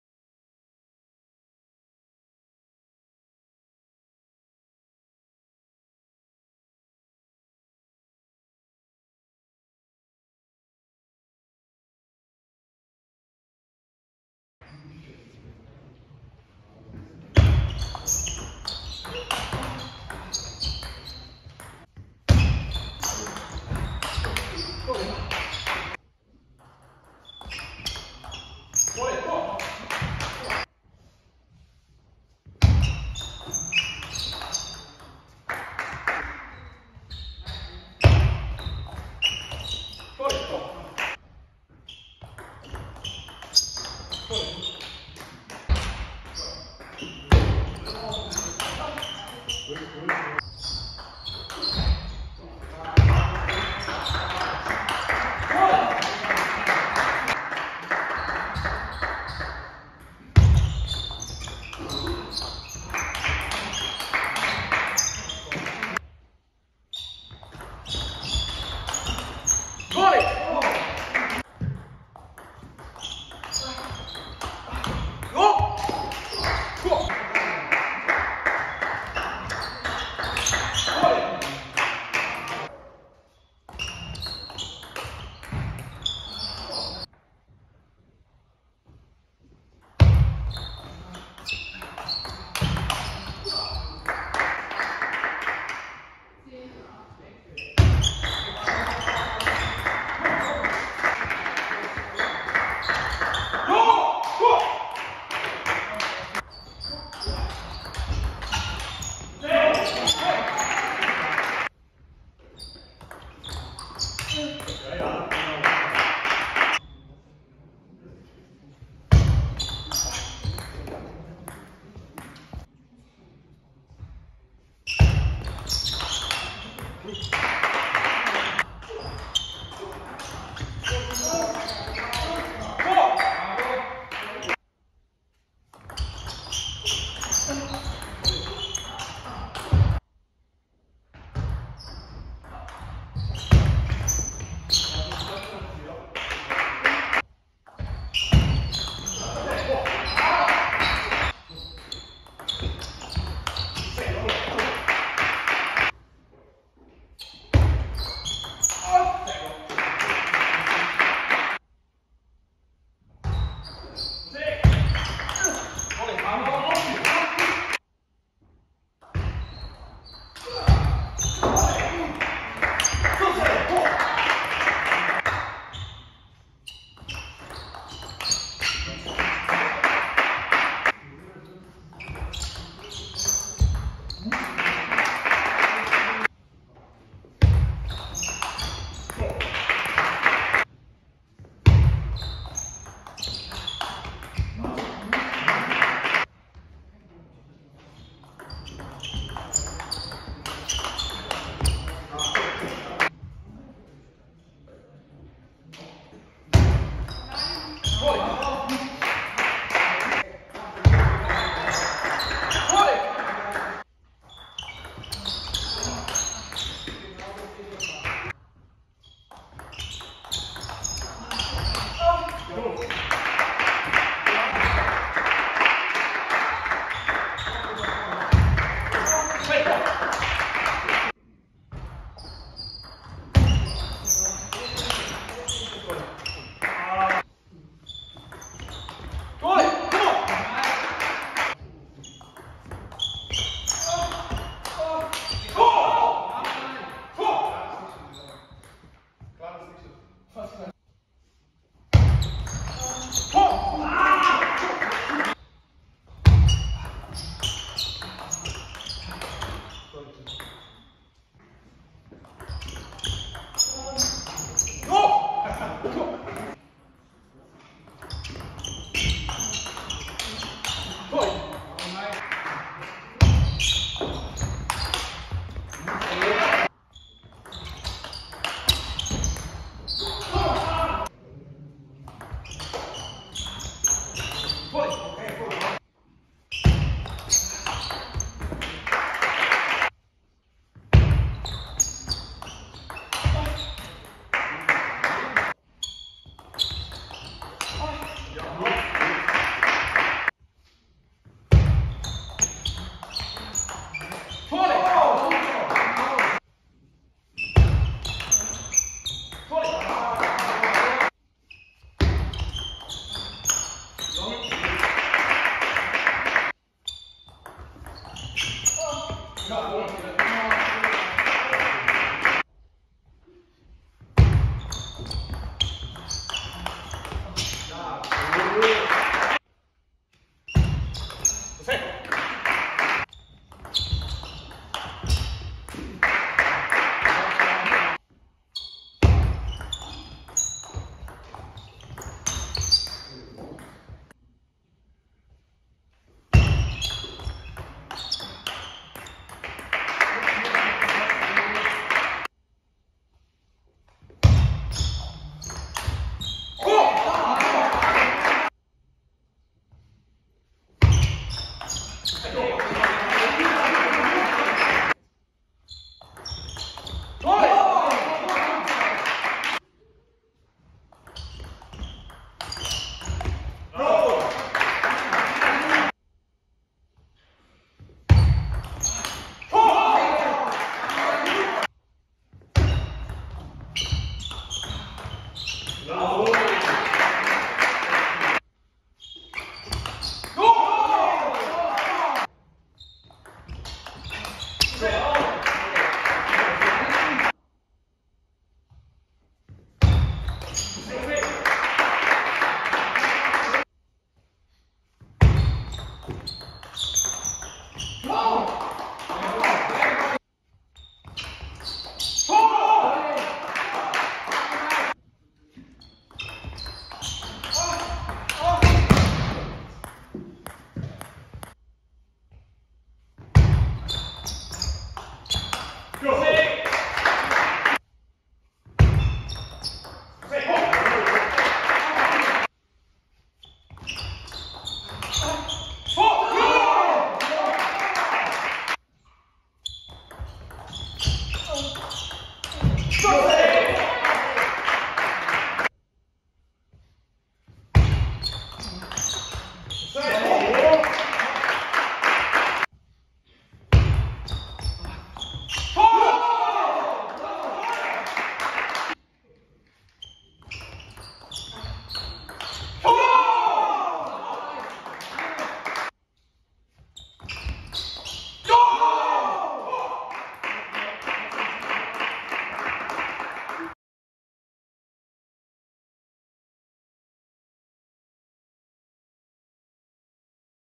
3.Bundesliga Süd | Mike Hollo(2214TTR) : Pavol Mego(2187TTR) Wer die meisten Fehler macht Verliert!
Ein sehr komischer Spiel verlauf 🙄

🆕Ihr wollt mich und @This is Table Tennis Germany supporten?